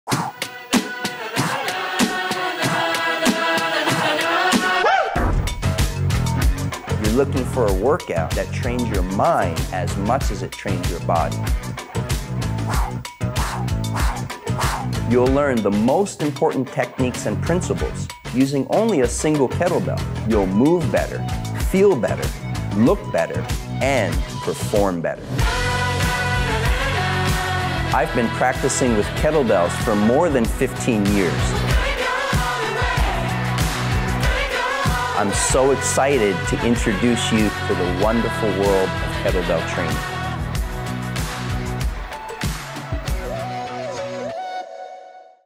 The Way of The Kettlebell by Steve Cotter
Ranked as Men’s Health Magazine’s Top 100 Fittest Men of All Time, if anyone can give you fitness tips, it’s Steve Cotter 💪 In this video, we’re bringing to you The 7 Day ‘Way of the Kettlebell’ at-home course 🏋🏼 Check it out and land your spot now

Here are a few of the reasons why this type of movement works:

💥 Increases skeleton strength
💥 Allows for unlimited range of motion to suit all body types
💥 Boosts power, endurance and flexibility in one

It’s one of the most incredible, not-so-secret hacks to health and fitness ever.

The Steve Cotter extreme kettlebell workout has already helped countless people get in the best shape of their lives...join our kettlebell tribe now!